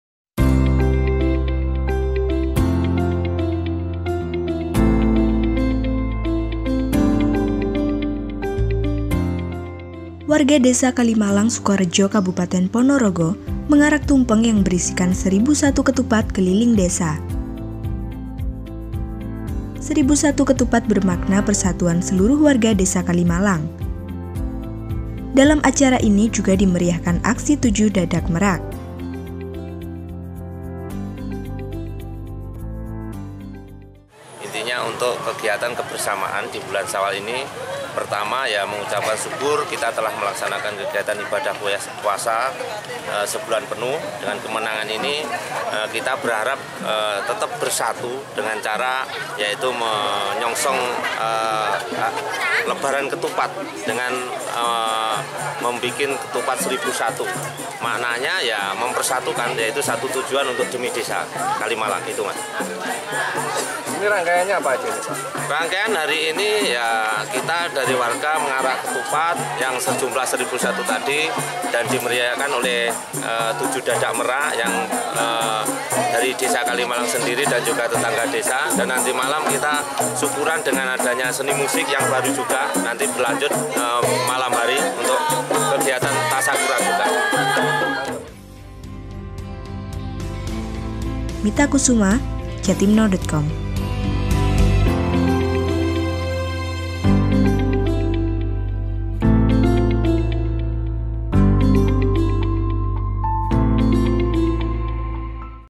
Kirab 1001 Tumpeng Ketupat di Ponorogo
Warga Desa Kalimalang, Sukorejo, Kabupaten Ponorogo mengarak tumpeng yang berisikan 1001 ketupat keliling desa.

1001 ketupat bermakna persatuan seluruh warga Desa Kalimalang. Dalam acara ini, juga dimeriahkan aksi tujuh dadak merak.